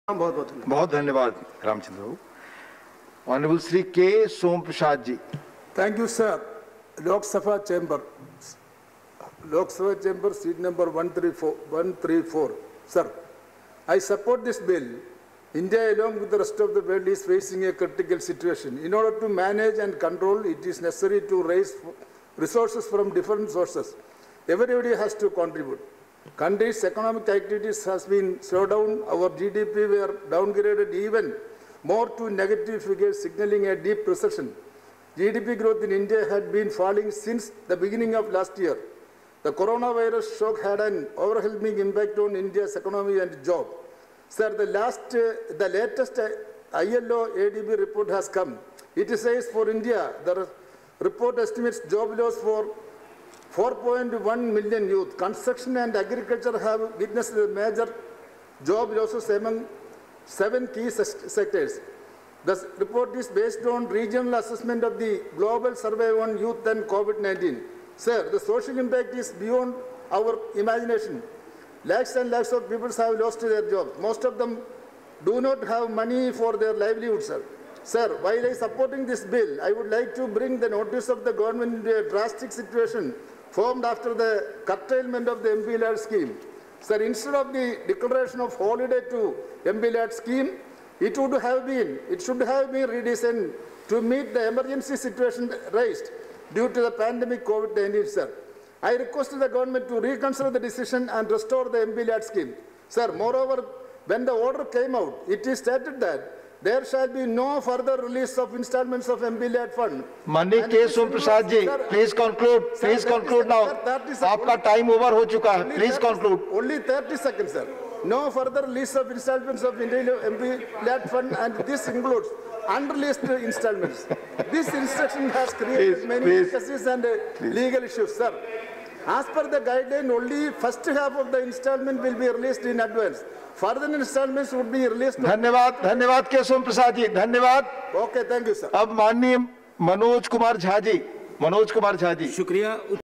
K. Somaprasad's Remarks | Salary, Allowances & Pension of MPs (Amend) Bill 2020
Discussion on the following bills:

The Salaries and Allowances of Ministers (Amendment) Bill, 2020 - bill further amends the Salaries and Allowances of Ministers Act, 1952.

The Salary, Allowances and Pension of Members of Parliament (Amendment) Bill, 2020 -  bill further amends the Salary, Allowances and Pension of Members of Parliament Act, 1954.